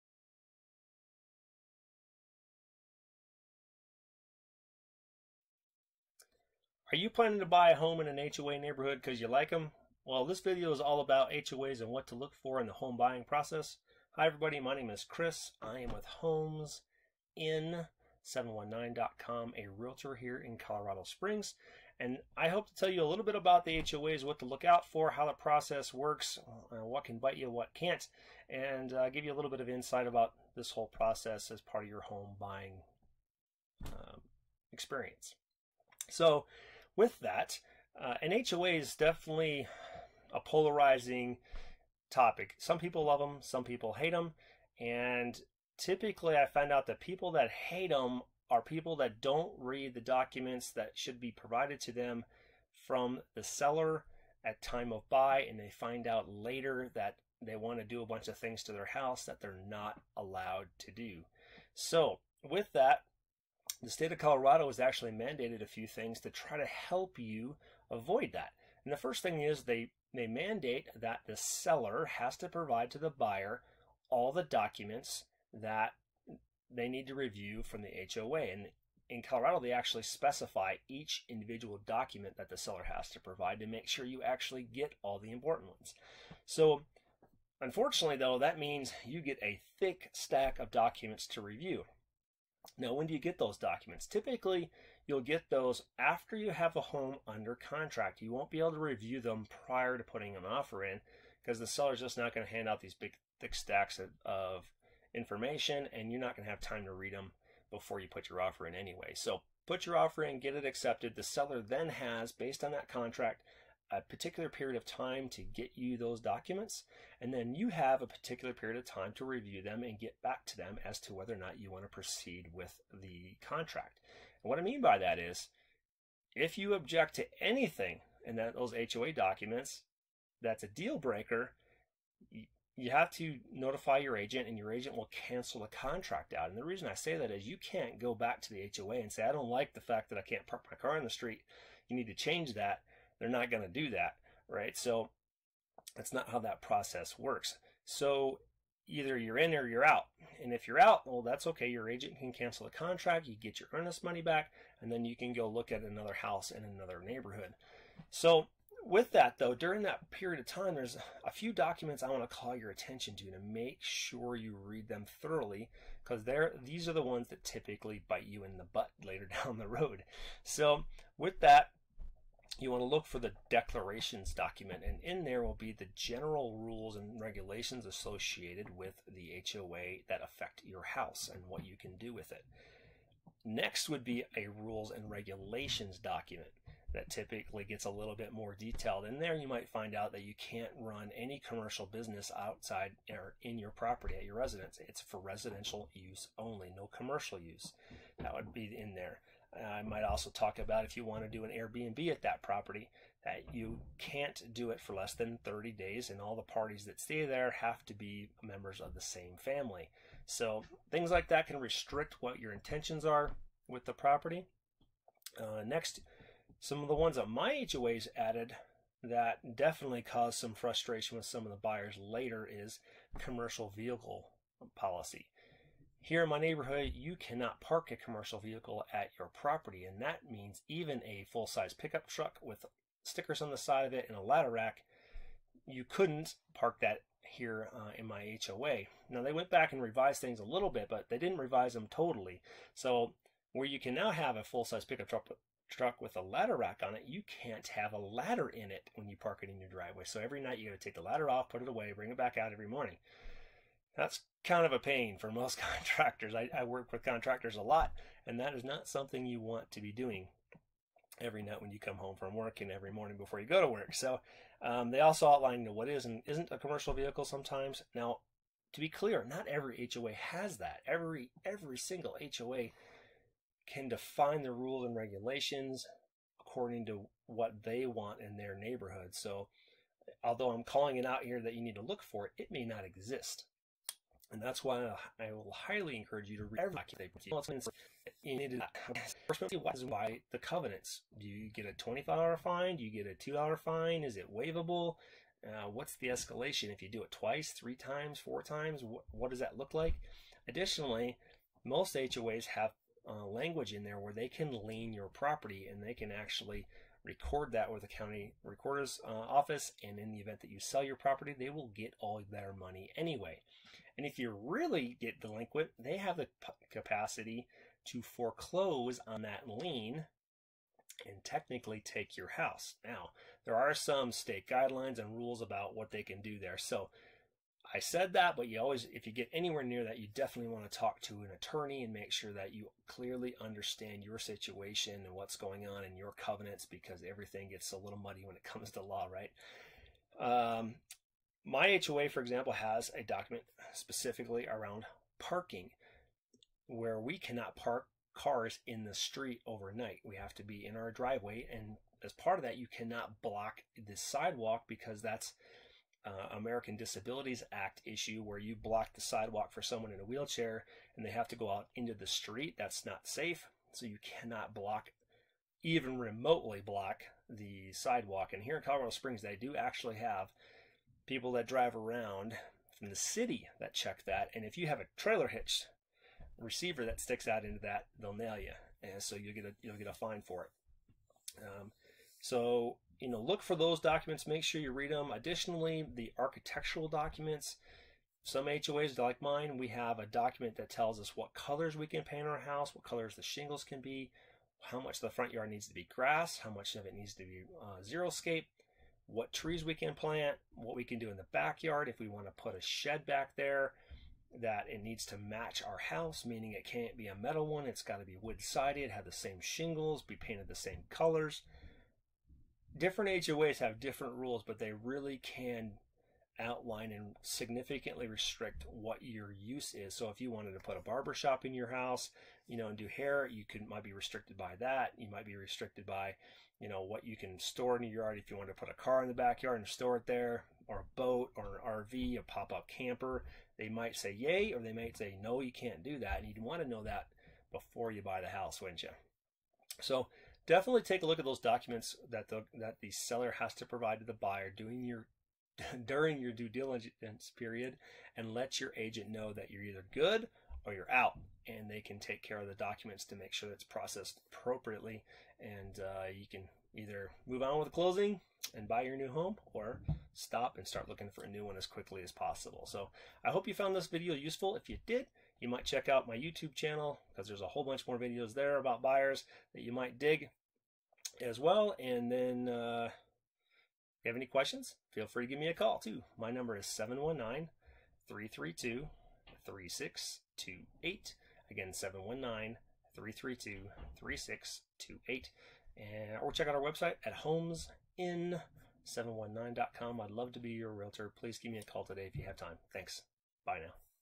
HOA's, what to know before you buy!
Home Owner Associations (HOA) are not for everyone because they can be very restrictive. Watch this video learn where more before you buy a home in one.